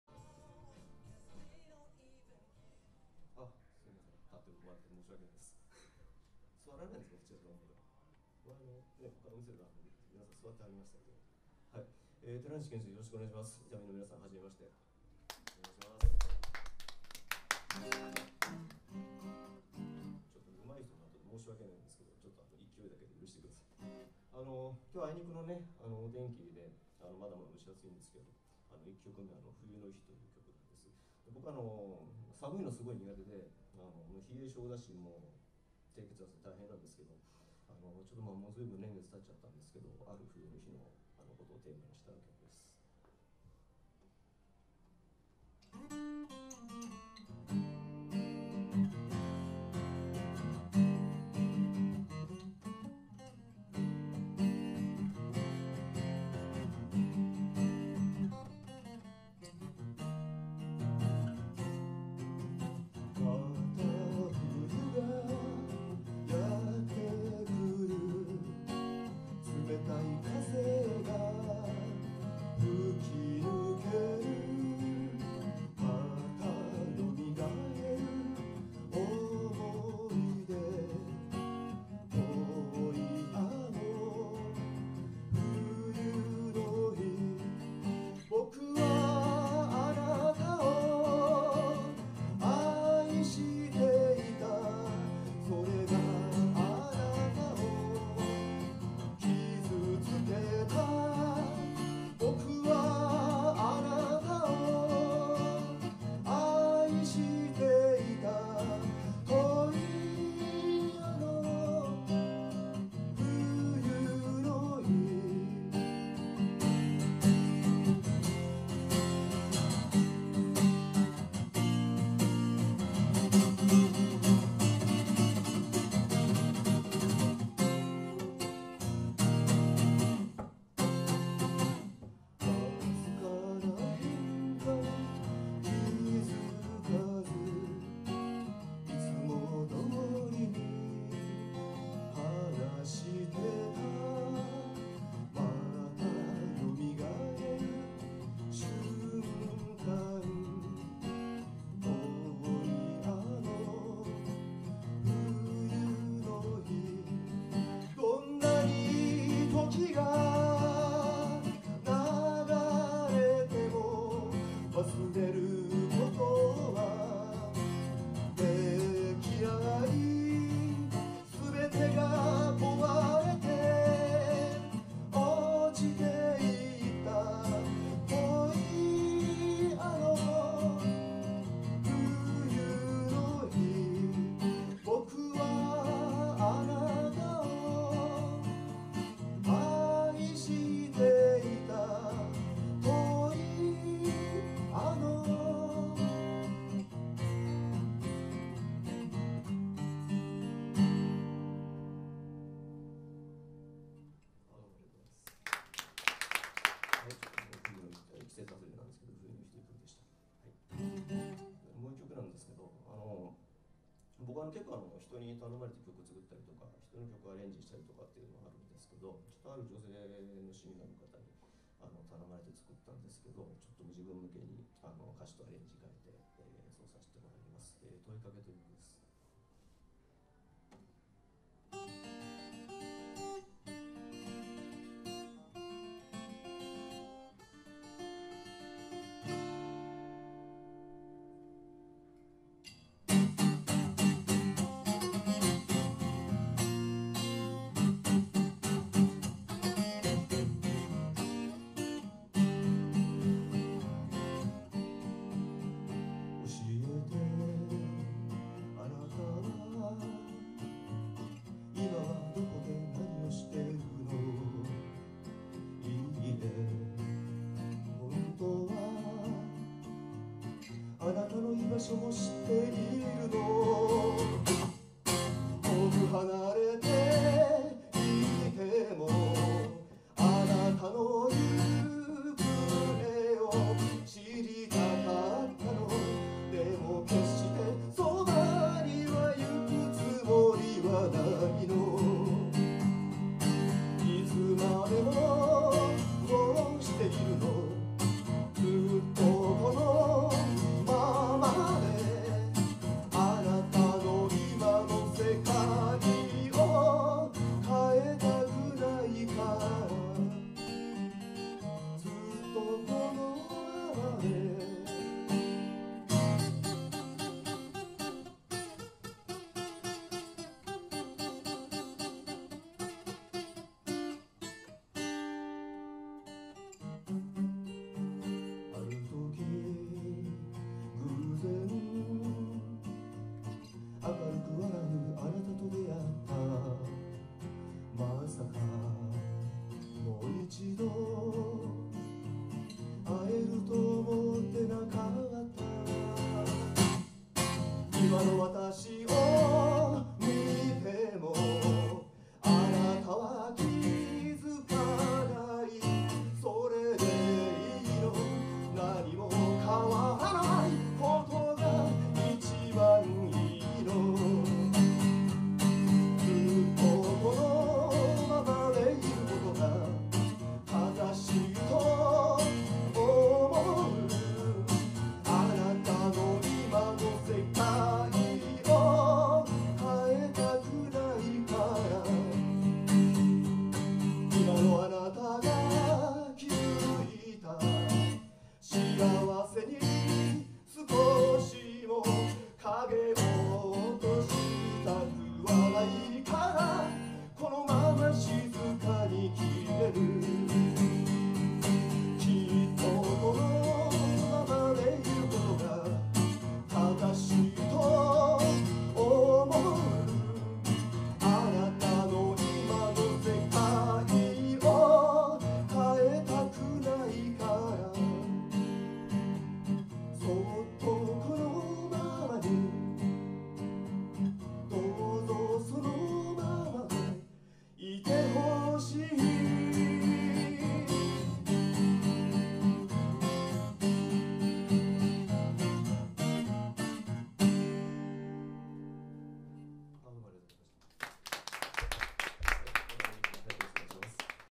寺西建二 HOTLINE2014 島村楽器伊丹昆陽店 店予選動画
島村楽器伊丹昆陽店で8月3日に開催された、HOTLINE2014店予選のレポート動画です。